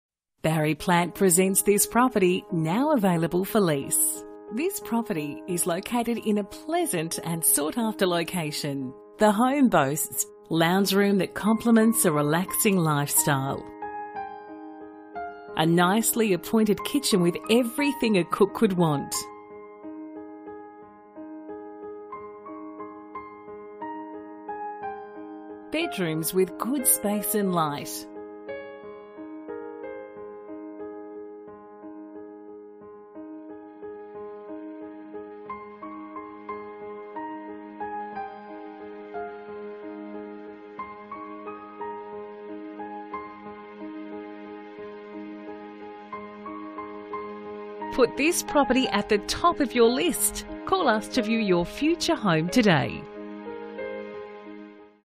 For Rent 75 Bowes Avenue Airport West Vic 3042 - English
For Rent -  75 Bowes Avenue Airport West Vic 3042 Barry Plant Niddrie 3 Bedrooms 1 Bathroom 2Garages   6 Month Lease This delightful home with gas ducted heating is in a most sought after pocket of Airport West within walking distance to tram, freeway and Westfield/Essendon is easily accessible. It offers three bedrooms master and second bedroom with robes , separate lounge and dining, central updated kitchen, tiled bathroom with separate shower, sunroom, large covered rear porch, carpet and garage. An inspection will give you a true appreciation of this house.